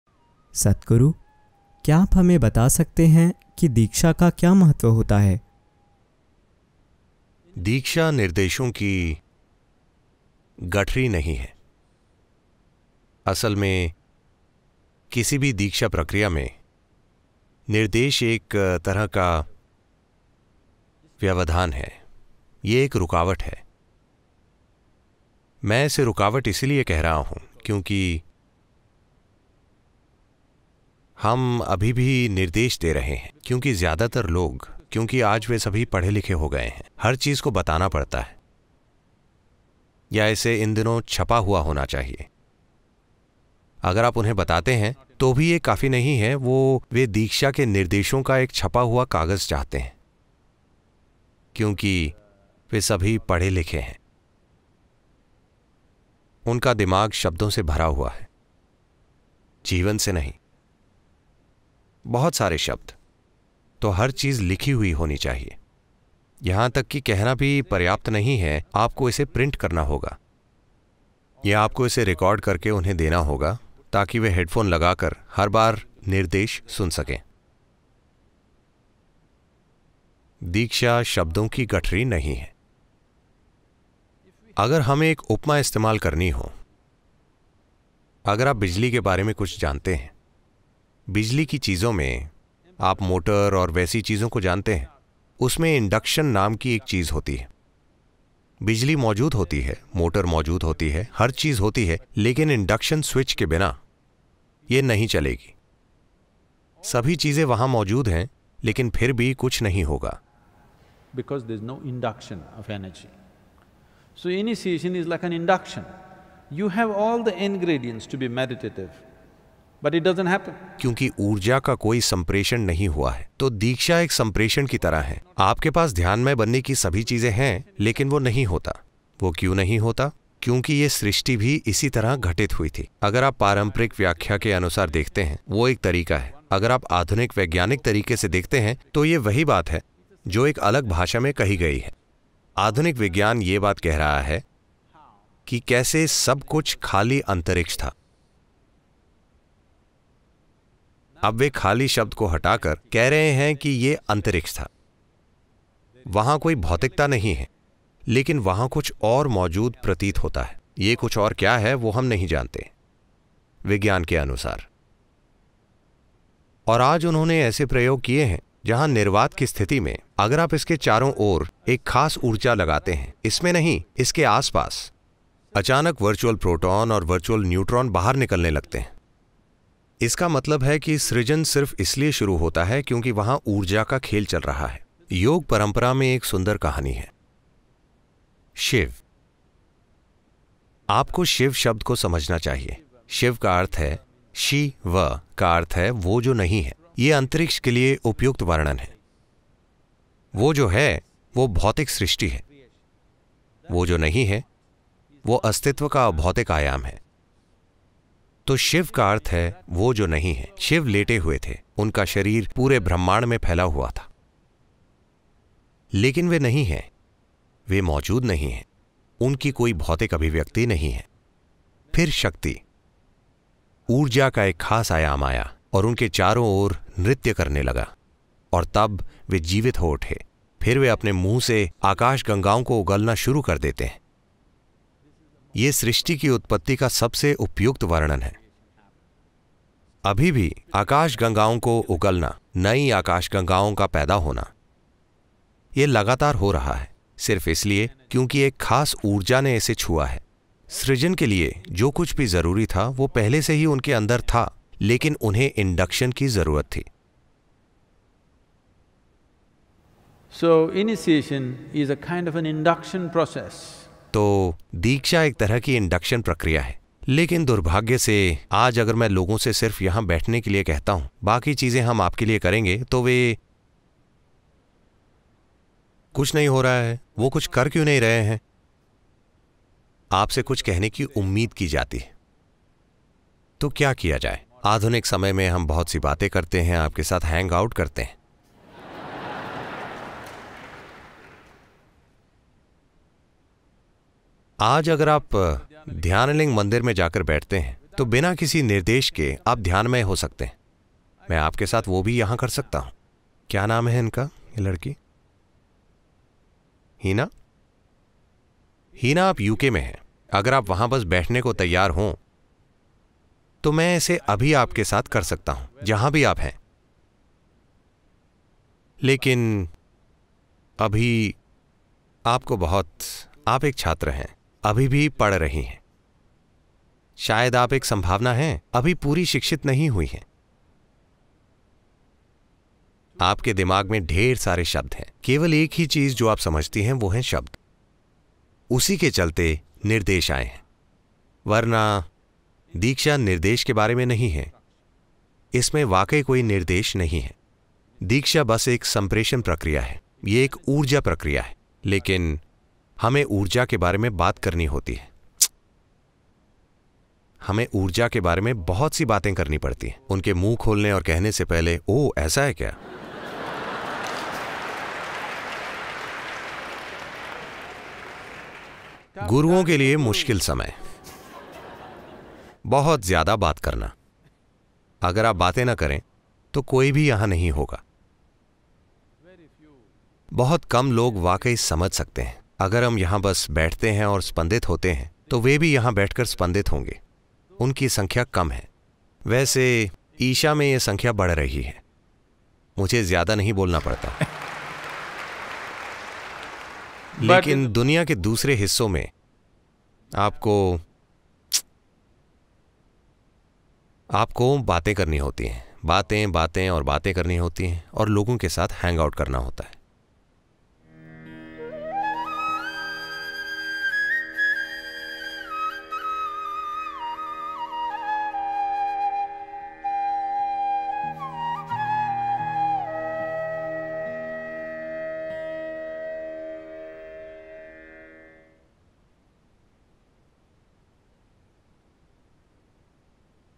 दीक्षा लेने से क्या होता है? | Sadhguru Hindi
ध्यान की दीक्षा क्यों दी जाती है? सद्गुरु दीक्षा का मतलब समझाते हुए कह रहे हैं कि दीक्षा एक ऊर्जा की प्रकिया है। ध्यान के लिए ज़रूरी हर चीज़ हमारे अंदर पहले ही मौजूद होती है, लेकिन उस दिशा में कदम उठाने के लिए दीक्षा की ऊर्जा ज़रूरी होती है।